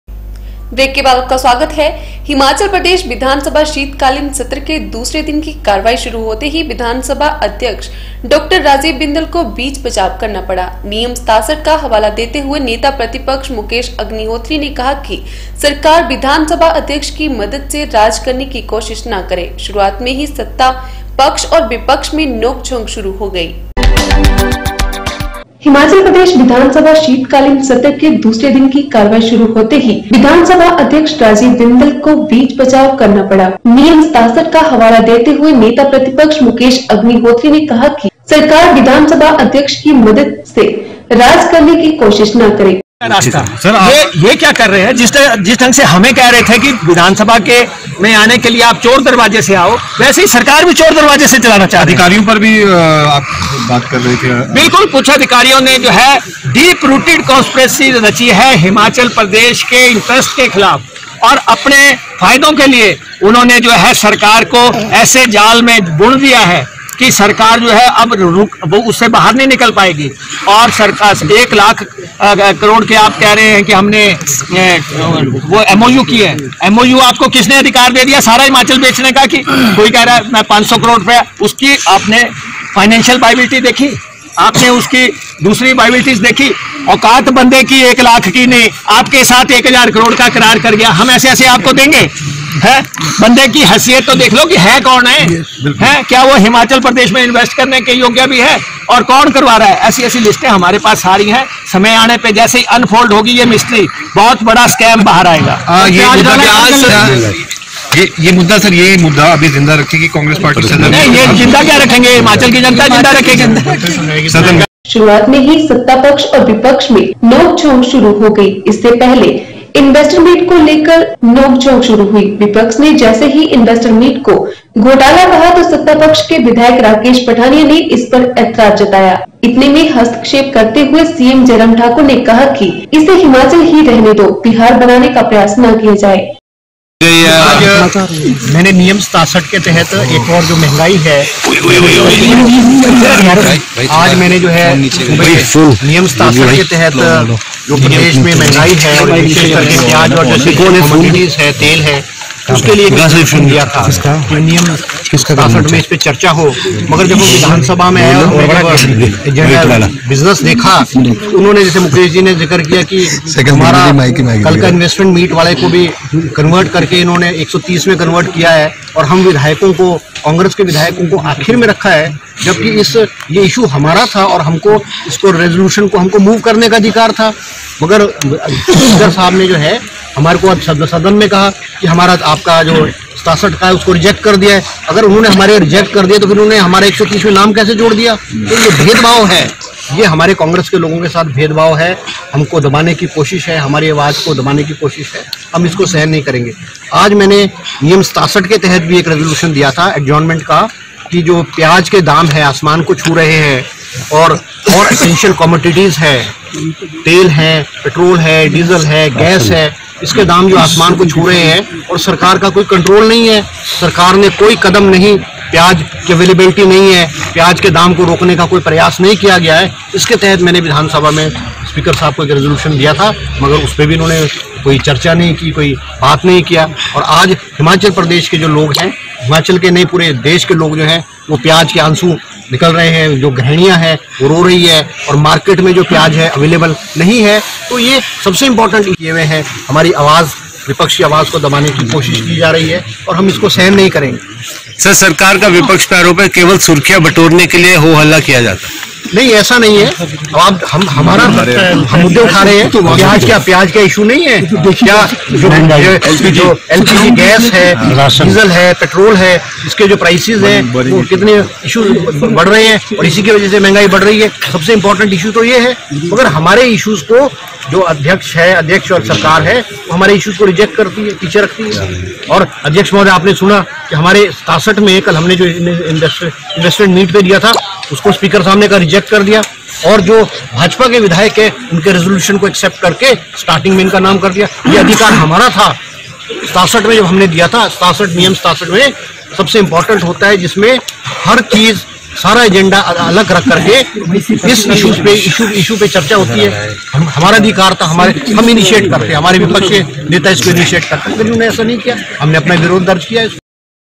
शीतकालीन सत्र के दूसरे दिन की कार्यवाही शुरू होते ही विधानसभा अध्यक्ष को बीच-बचाव करना पड़ा
हिमाचल प्रदेश विधानसभा शीतकालीन सत्र के दूसरे दिन की कार्यवाही शुरू होते ही विधानसभा अध्यक्ष डॉ राजीव बिंदल को बीच-बचाव करना पड़ा। नियम 67 का हवाला देते हुए नेता प्रतिपक्ष मुकेश अग्निहोत्री ने कहा कि सरकार विधानसभा अध्यक्ष की मदद से राज करने कि कोशिश ना करें। शुरुआत में ही सत्तापक्ष और विपक्ष में नोंकझोंक शुरू हो गई।